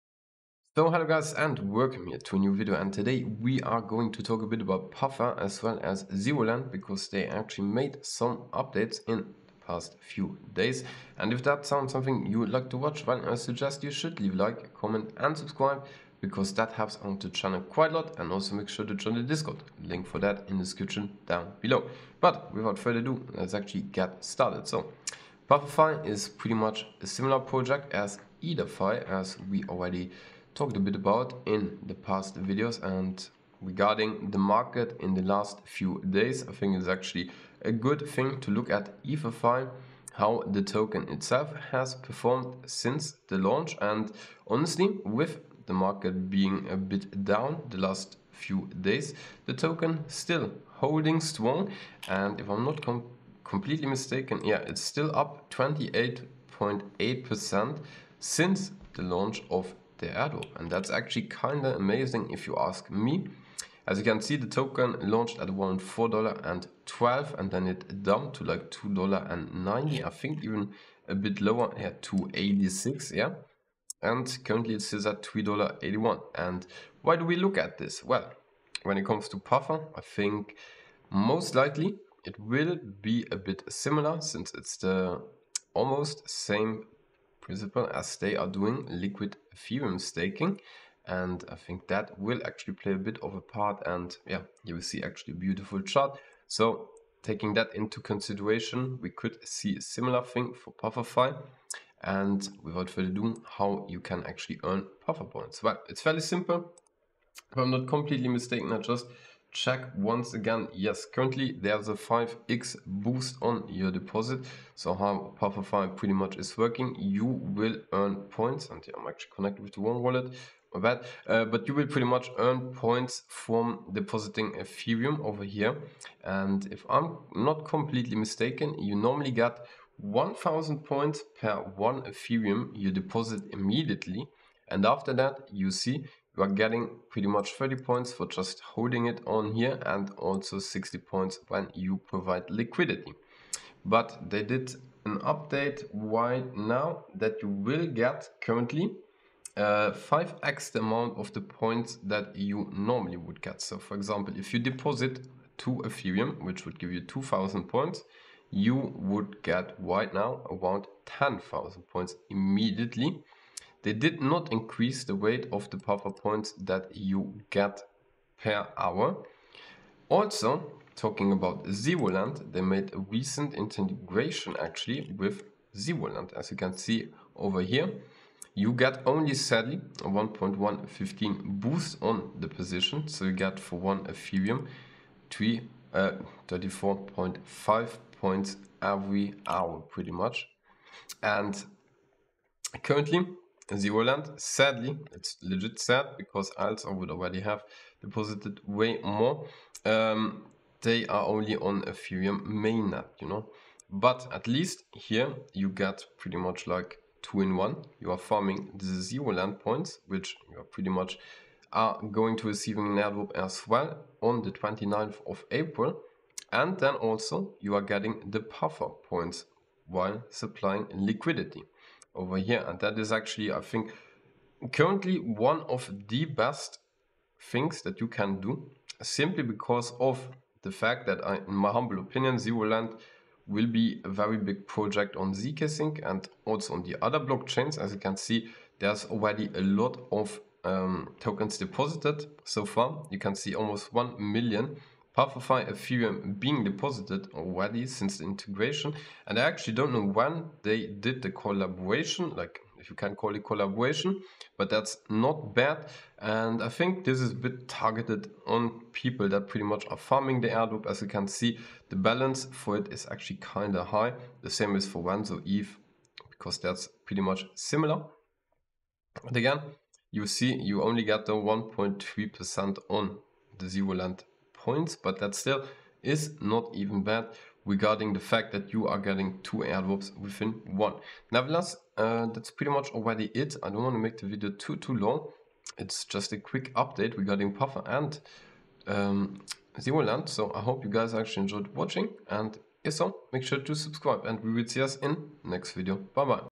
Puffer Finance Airdrop Updates! ZeroLend Integration!
So in todays video we are going to talk about the new intergration of puffer which is zerolend. we are having a look on how you can farm both airdrops at one!